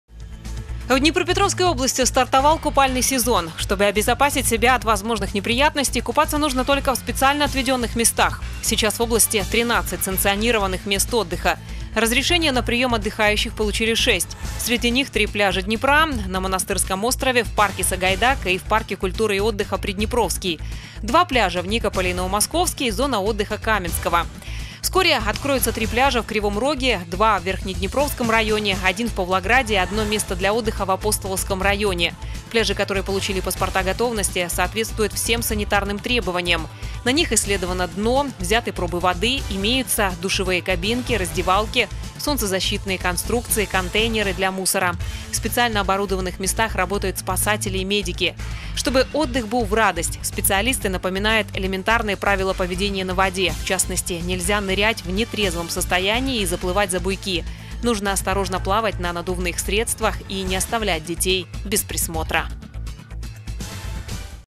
Где безопасно купаться в летнюю жару?
В Днепропетровской области стартовал купальный сезон. Чтобы обезопасить себя от возможных неприятностей, купаться нужно только в специально отведенных местах.